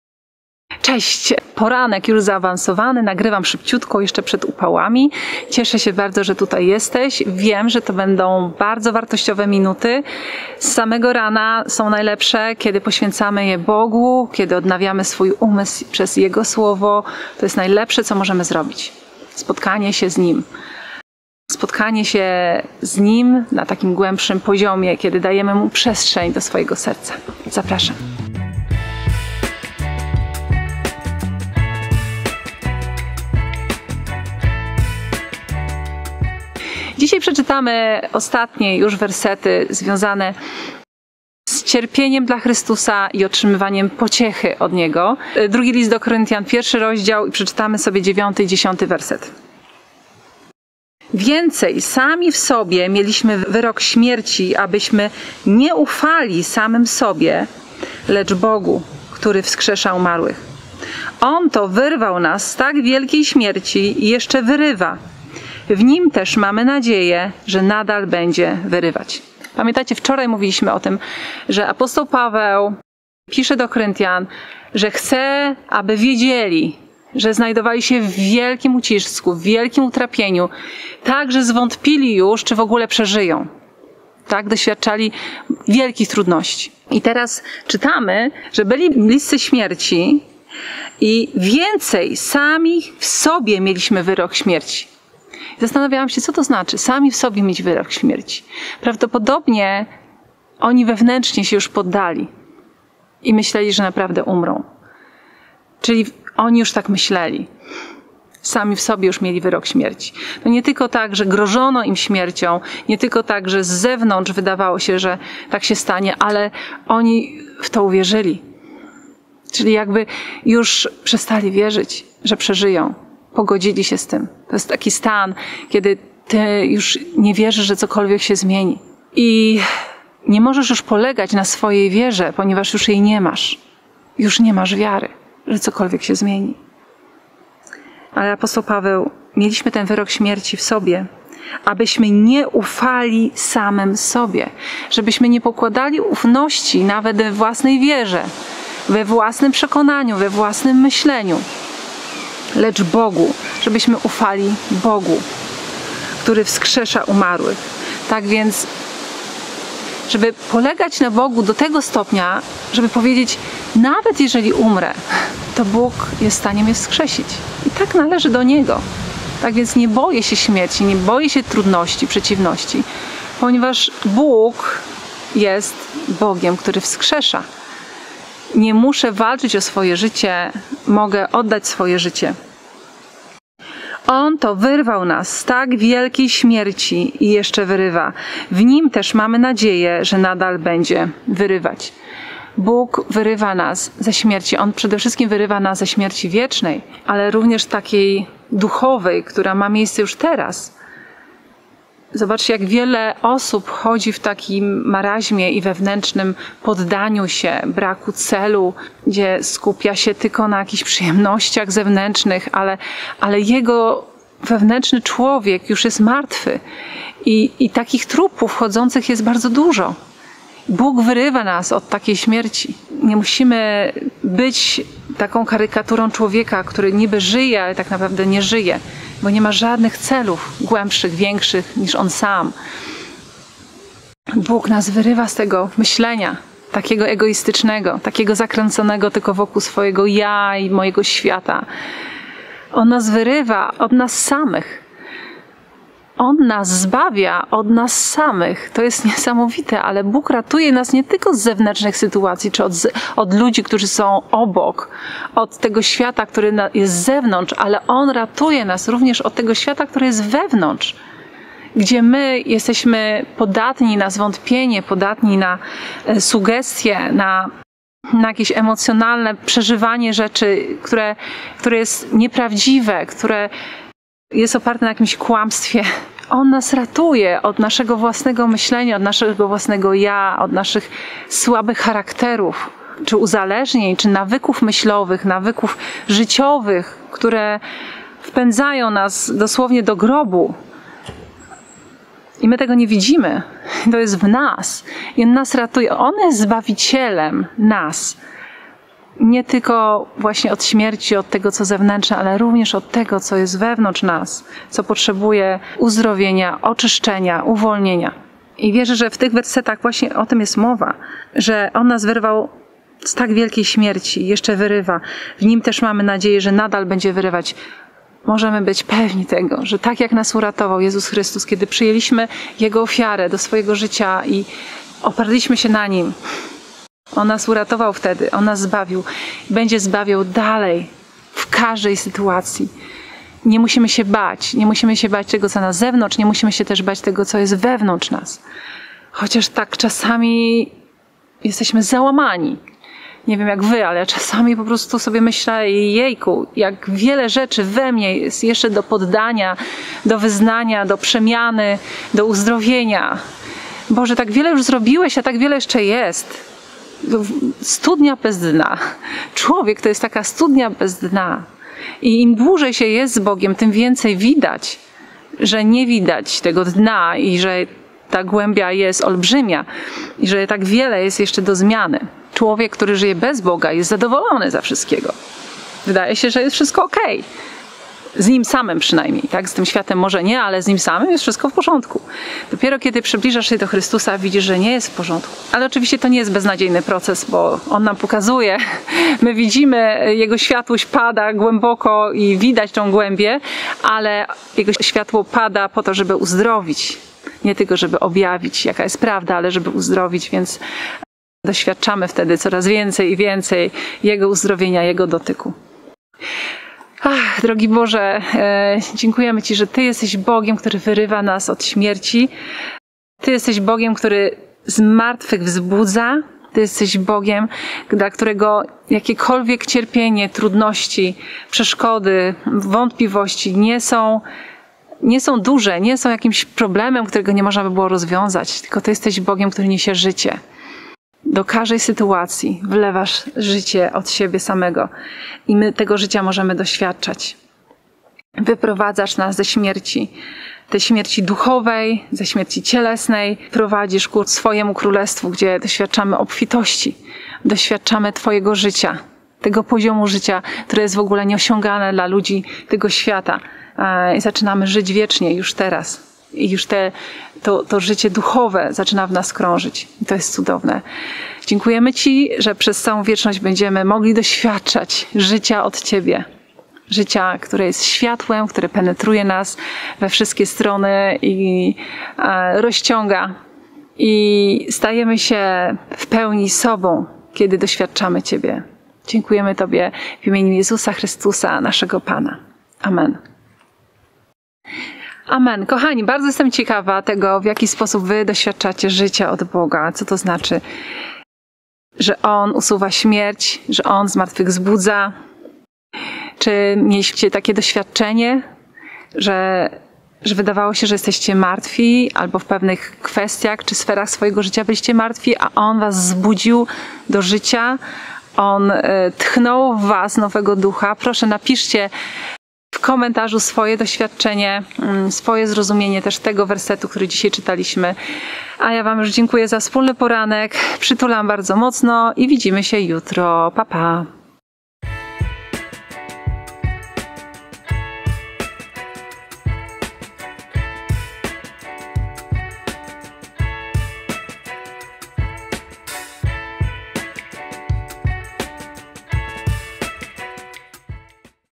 Nie ufaj sam sobie! [5 Rano #1150]
„Więcej, sami w sobie mieliśmy wyrok śmierci, abyśmy nie ufali samym sobie, lecz Bogu, który wskrzesza umarłych. On to wyrwał nas z tak wielkiej śmierci i jeszcze wyrywa. W nim też mamy nadzieję, że nadal będzie wyrywać;”
‭‭II Koryntian‬ ‭1‬:‭9‬-‭10‬